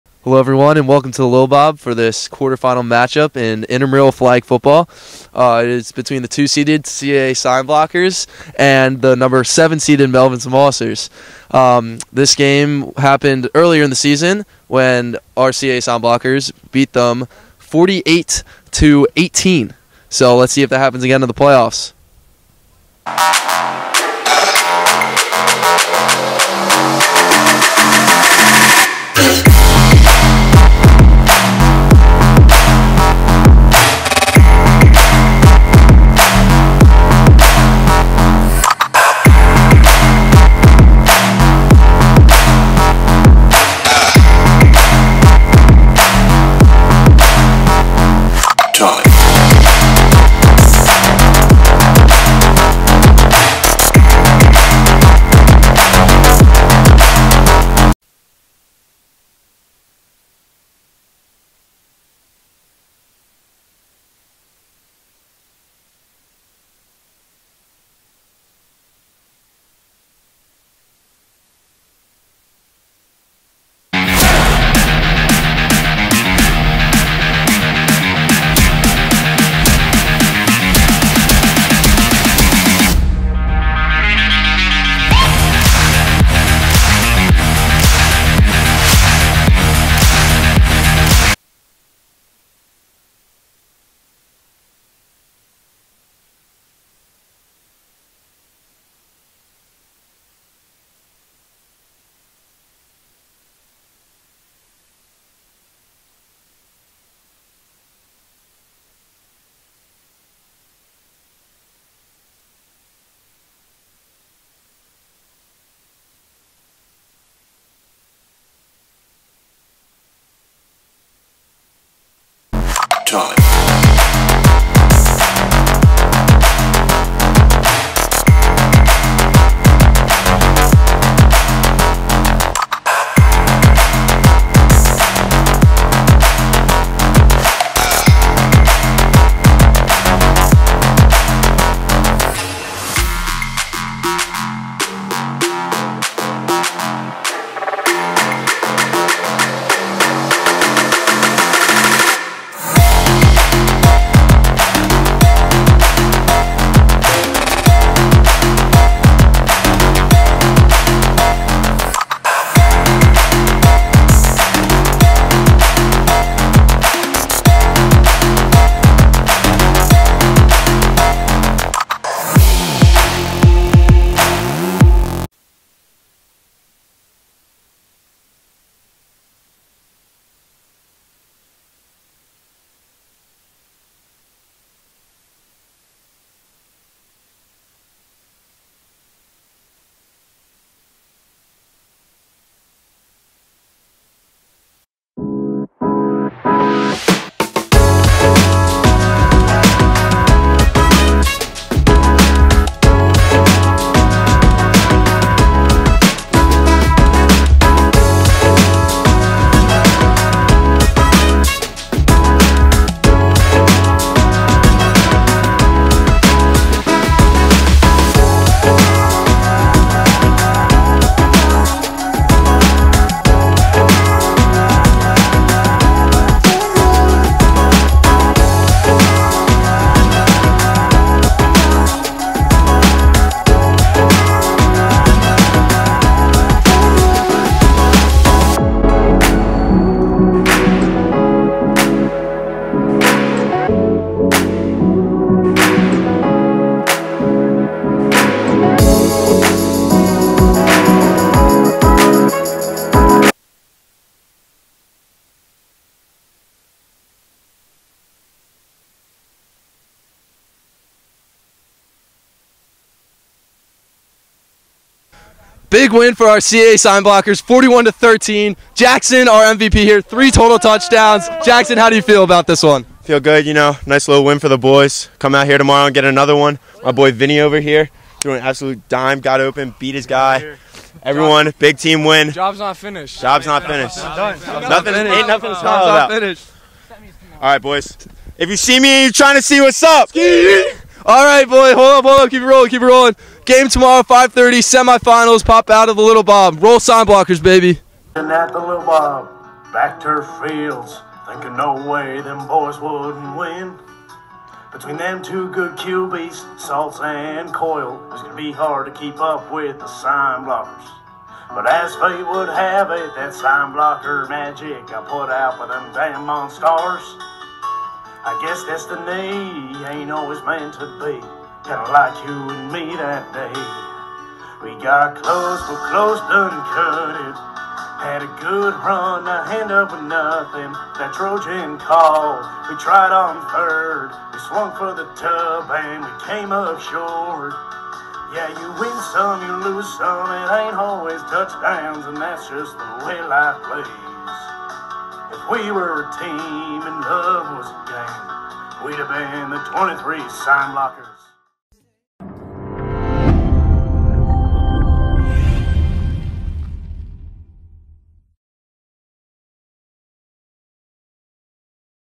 Ryan Furman's Flag Football Highlights
Ryan Furman and his team, the CAA Sign Blockers, face the Monstarz in the flag football playoffs at the University of Delaware!